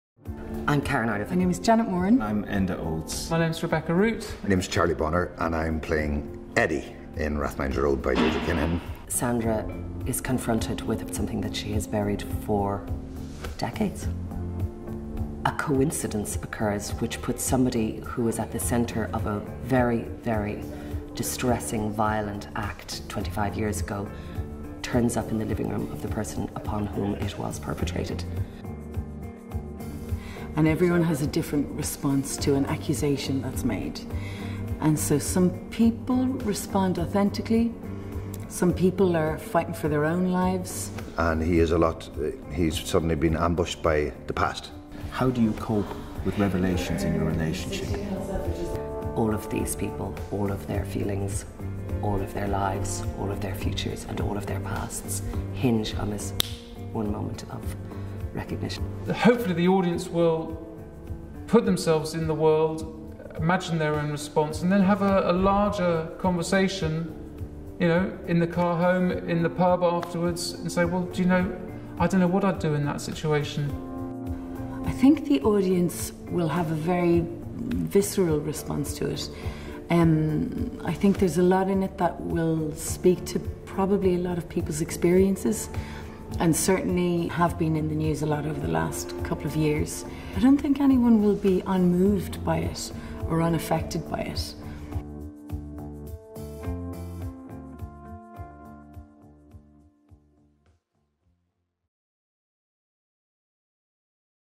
Rathmines Road | Meet the Cast
Rathmines Road, by Deirdre Kinahan, a co-production between Fishamble: The New Play Company and the Abbey Theatre.

9 - 27 October on the Peacock Stage as part of Dublin Theatre Festival